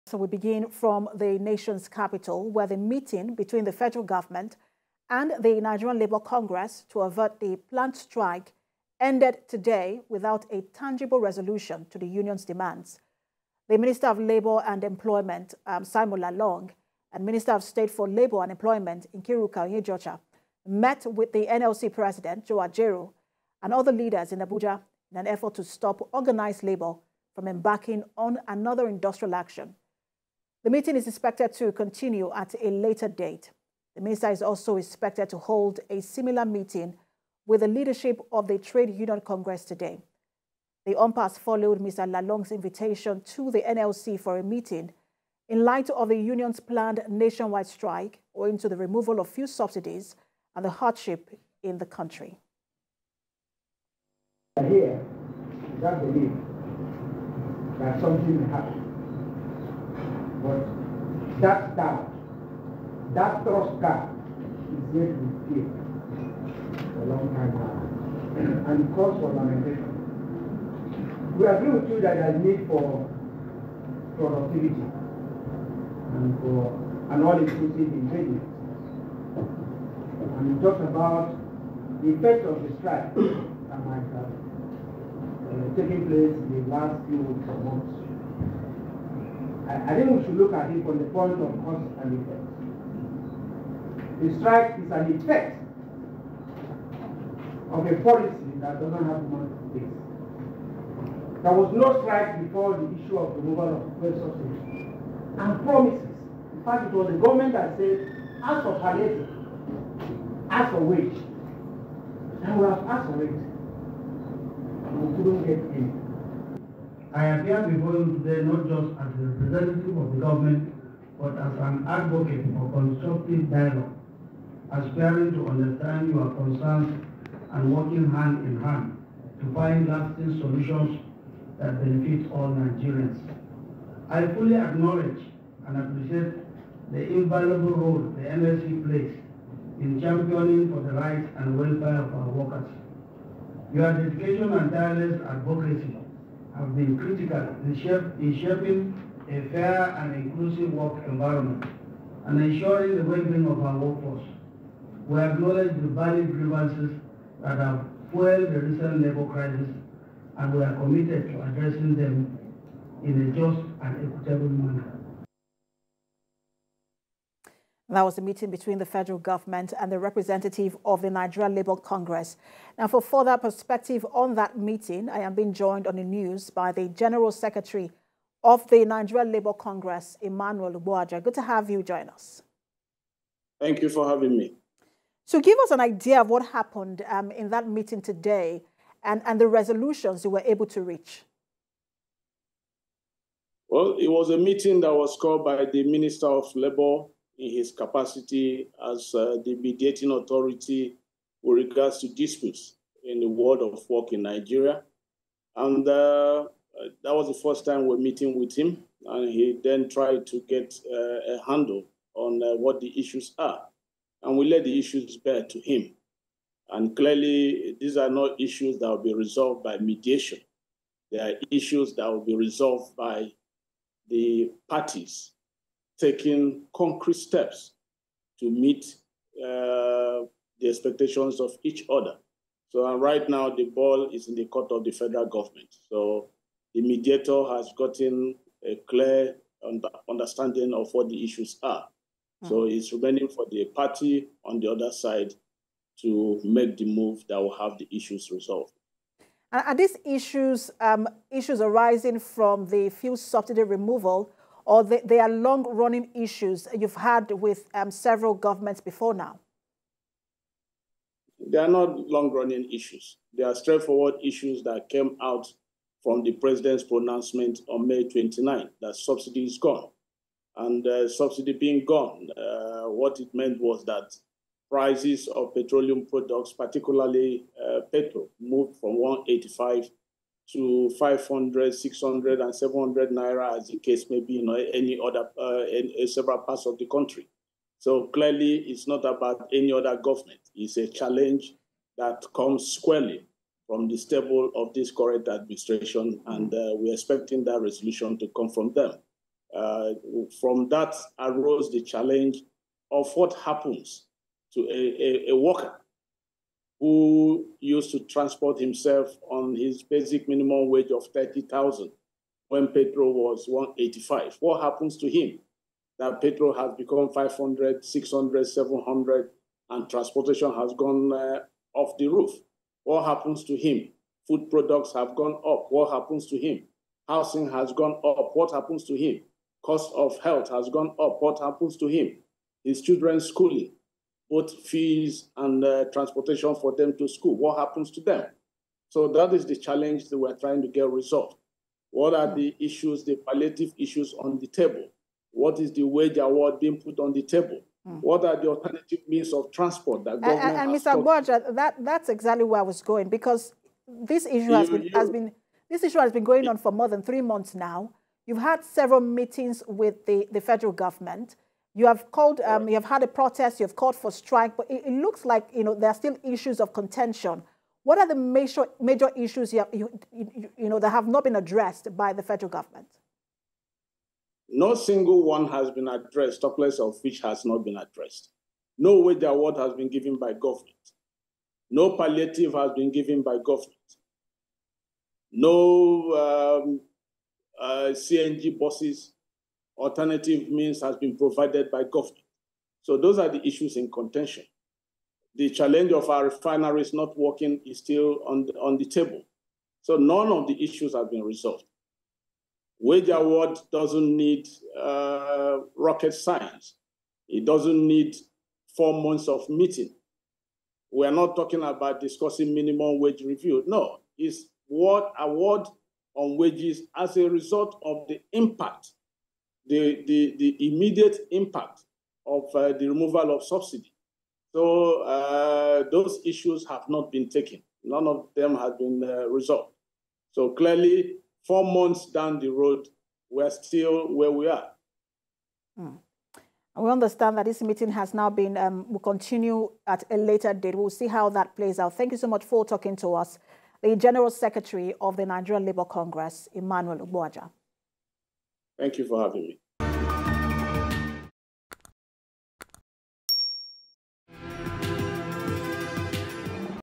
FG, NLC Meeting Ends Without Resolution
We begin from the nation’s Capital where the meeting between the Federal Government and the Nigeria Labour Congress to avert the planned strike ended on Monday without a tangible resolution to the union’s demands.
The Minister of Labour and Employment, Simon Lalong; and Minister of State for Labour and Employment, Nkeiruka Onyejeocha, met with the NLC President, Joe Ajaero, and other leaders in Abuja in an effort to stop organized labour from embarking on another industrial action.
The meeting is expected to continue at a later date.
The minister is also expected to hold a similar meeting with the leadership of the Trade Union Congress today.